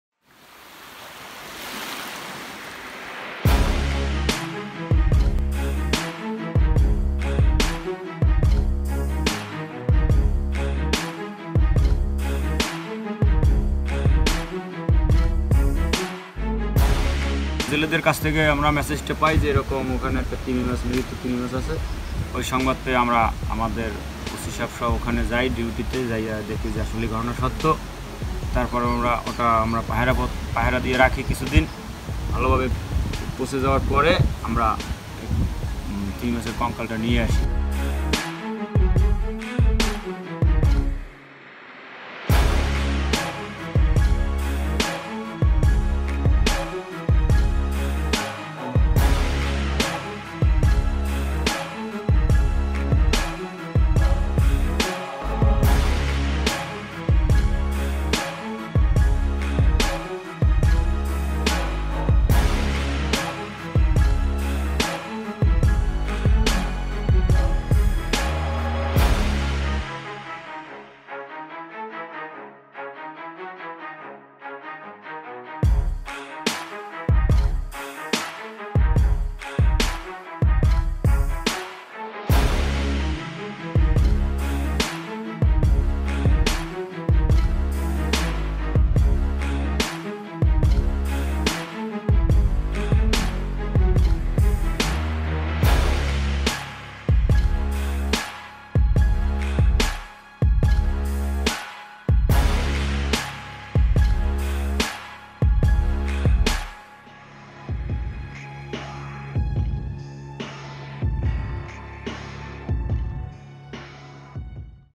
Dead whales washing ashore, Bangladesh
The Kotka beach is mostly empty, save plastic bottles and trash that washed ashore, explaining our shock at finding a skeleton almost 40-foot long of a whale in Kotka of Sundarbans. Now being kept at a dilapidated house of the forest guard office, the skeleton is so huge that the head alone has to be hoisted by four people. While the head lies on the veranda the rest of the skeleton is inside the tin-shed building.

Experts could not yet officially identify the species. After inspecting photos of the whale, Rubaiyat M Mansur, country representative of Wildlife Conservation Society (WCS), said it could have been a Baleen whale.

“It is difficult to determine the species from just a photo. But it is possibly a Baleen whale. This type of whale can be found in the Swatch of No Ground ,” he said.
In August, last year, while patrolling the area, a team of forest guards found the dead whale near Kotka beach. At that time the rotting process had already started. The forest officials directed them to guard the whale and collect the bones from the beach. “We guarded it for a month so that no one could steal anything from it. Carrying it to this building was a difficult task for all of us. More than 10 people had to help us at bringing it to Kotka.”

In the last 10 months two whales have stranded on our coast—this one in Kotka and another one in Kuakata. . Last month fishermen found a 45-foot-long and 20-foot-wide marine mammal near the Kuakata eco-park area. These frequent stranding in a short span of time is a possible indicator that the ocean at our door step is rich in marine resources. Rubaiyat M Mansur said that they have found a pod of whales 42km deep inside the ocean from Cox’s Bazar coast, “We were amazed to see that there are whales near Cox’s Bazar. It means our cost is reach in marine mammals.”

Connect with The Daily Star Online